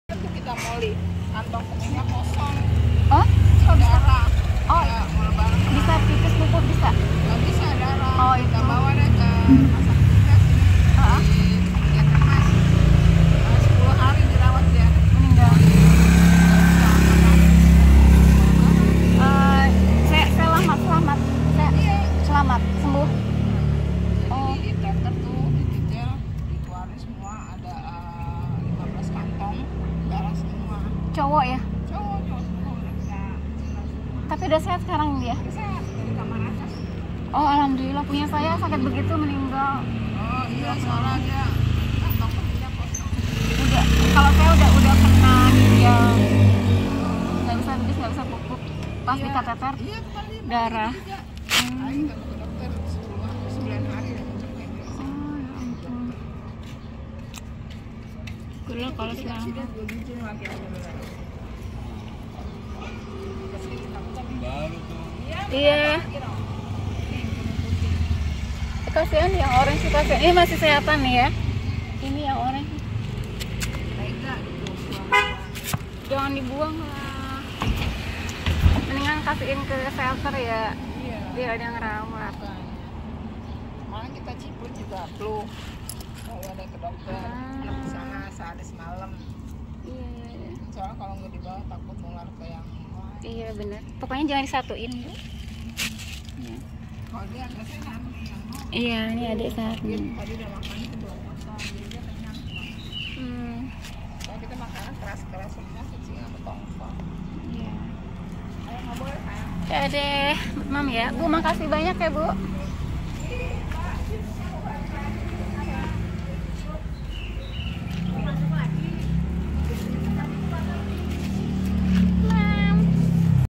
KASIAN deh Ade kecil Cemong ini dibuang orang dijalanan😡😡😡
Korban pembuangan kucing  ..aito lukasih makan kasian ni bocah linglung di jalan krn dibuang org😥